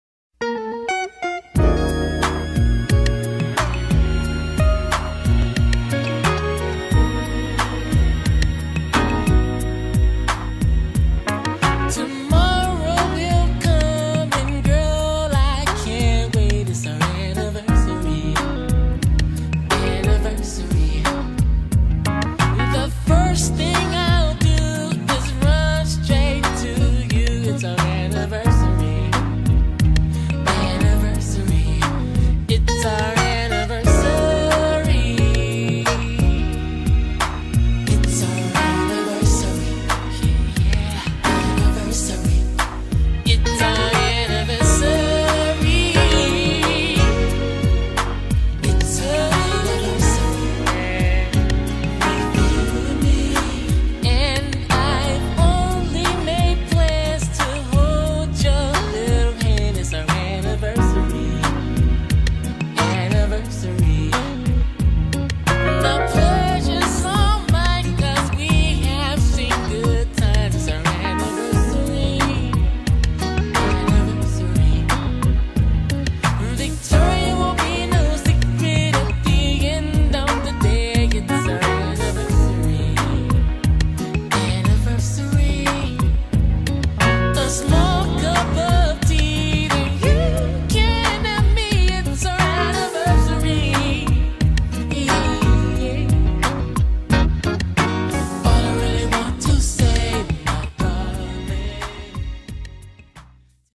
Husband uses ps3 to make anniversary slide show
Husband uses playstation 3 to  create slide show for wife